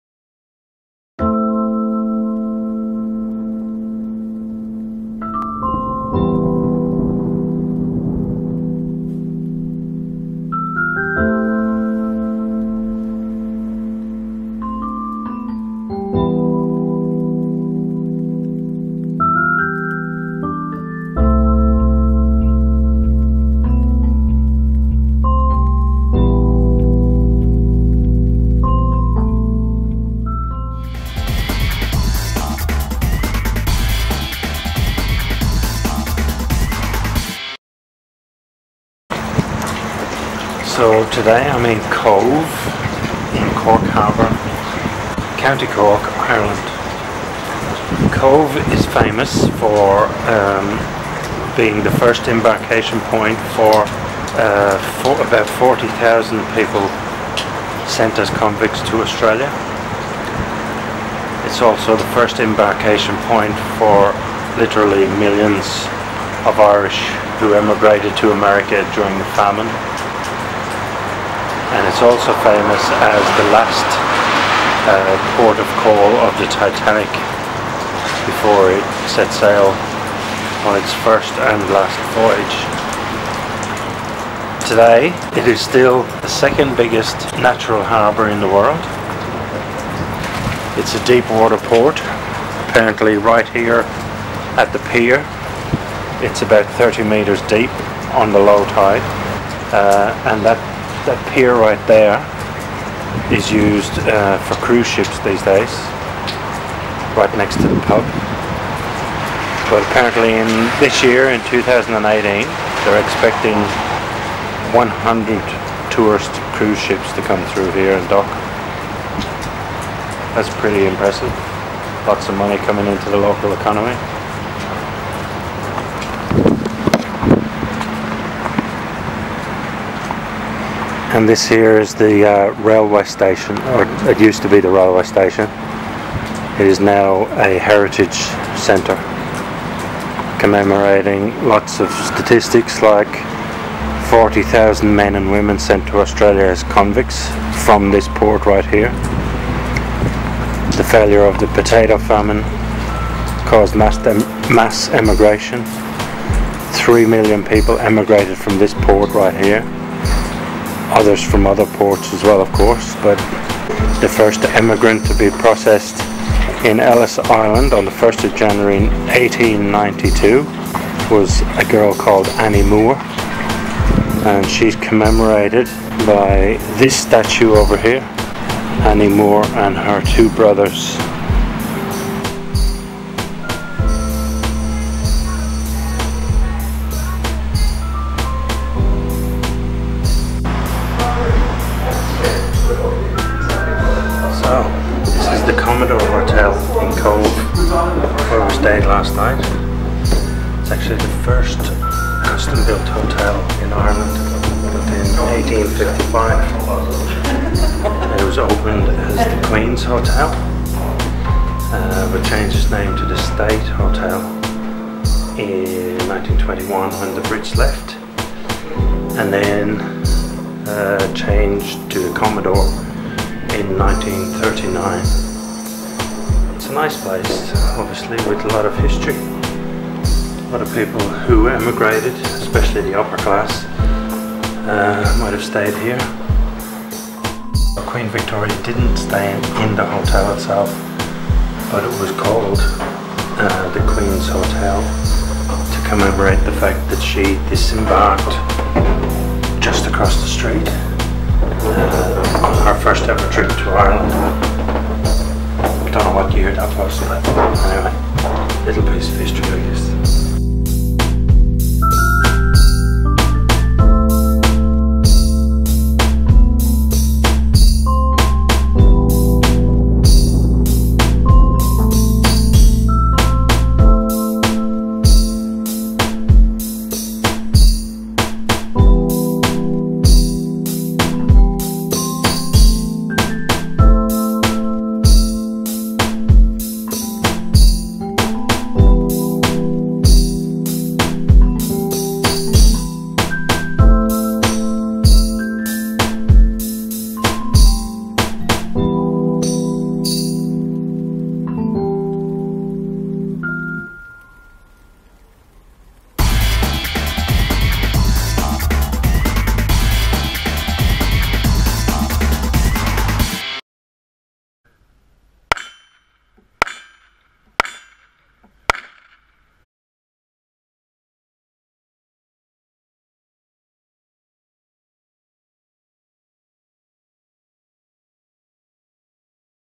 Nice overview of Cobh – ruined by boring narrator
Two fabulous days spent in Cobh, County Cork, Ireland.
This video is a simple overview of the town itself – with some nice imagery, but accompanied by the most boring narration (sorry about that – but please forgive me, I'm still learning).

Brian Hartigan
Managing Editor

Minnamurra
NSW 2533
AUSTRALIA